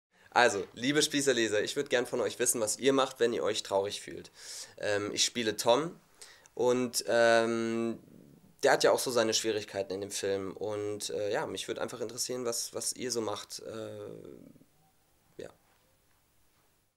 François Goeske fragt die SPIESSER-Leser
François Goeske stellt seine Frage an die SPIESSER-Leser.